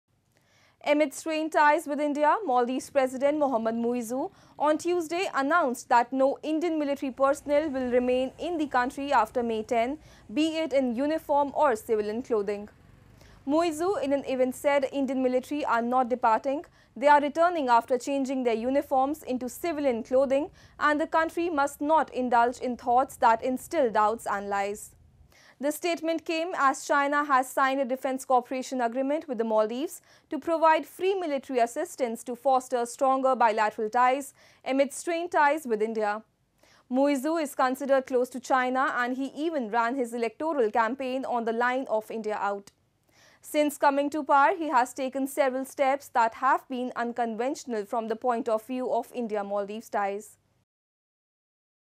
No Indian troops to remain in Maldives, not even in civilian clothing: President Muizzu
Amid strained ties with India, Maldives President Mohamad Muizzu on Tuesday announced that no Indian military personnel will remain in the country after May 10 - be it in uniform or civilian clothing. Muizzu in an event said Indian military are not departing, they are returning after changing their uniforms into civilian clothing, and the country must not indulge in thoughts that instill doubts and lies. The statement came as China has signed a defence cooperation agreement with the Maldives to provide free military assistance to foster "stronger" bilateral ties amid strained ties with India. Muizzu is considered close to China and he even ran his electoral campaign on the line of 'India Out'. Since coming to power, he has taken several steps that have been unconventional from the point of view of India-Maldives ties.